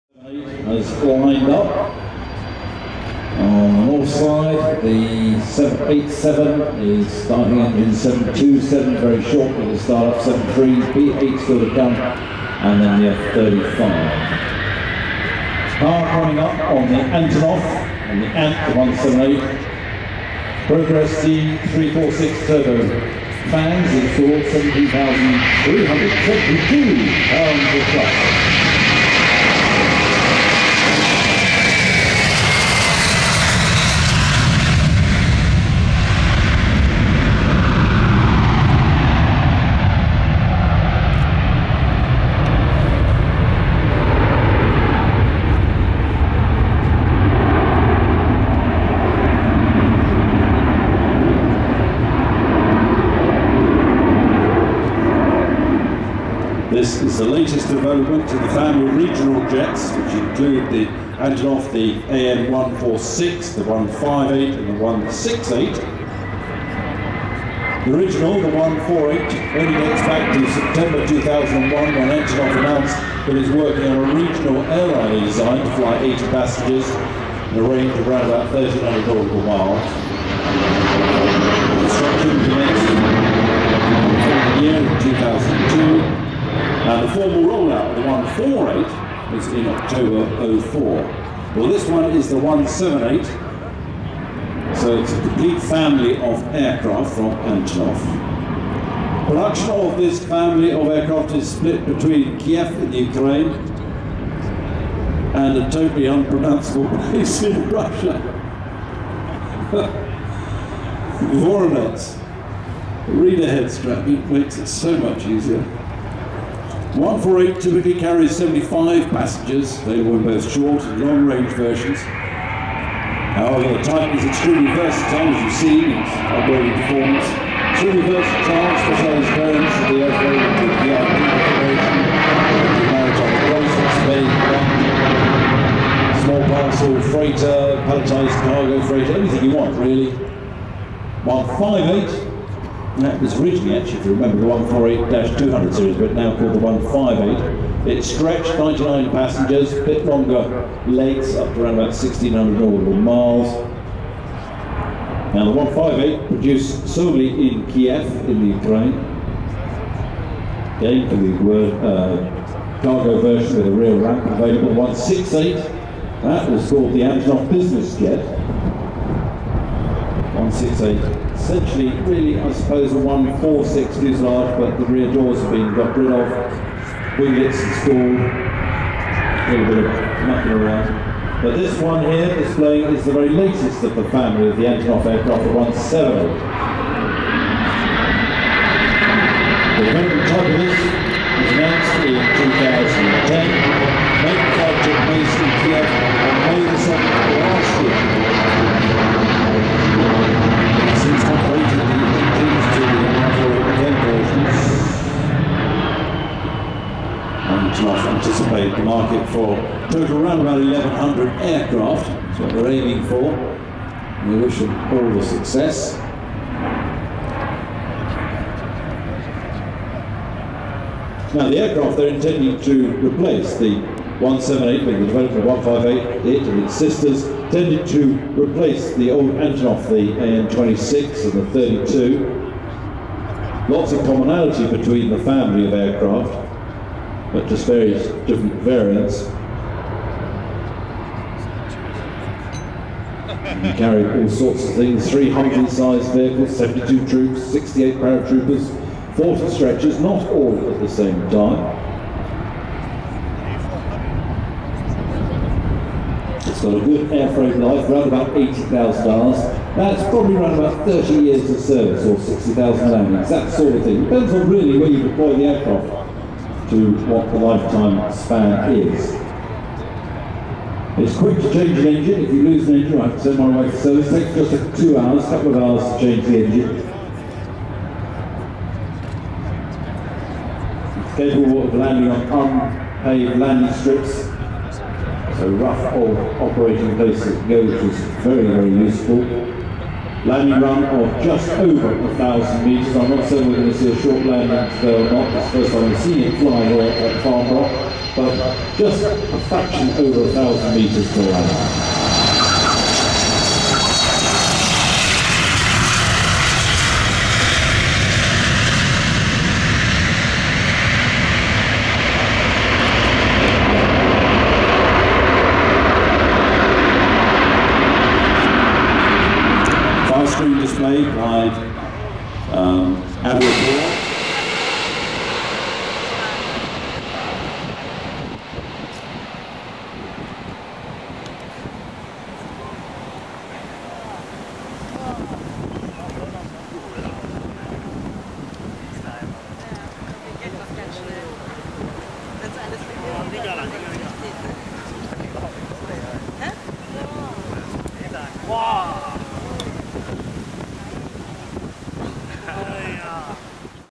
Antonov An-178 Flight Demonstration - Farnborough Airshow
Flight Demonstration - Trade Tuesday Farnborough Airshow 2016

The Antonov An-178 (Ukrainian: Антонов Ан-178) is a short-range medium-airlift military transport aircraft designed by the Ukrainian Antonov company and based on the Antonov An-158 (An-148-200). It was announced on 5 February 2010, rolled out on 16 April 2015 and the first flight was on 7 May 2015.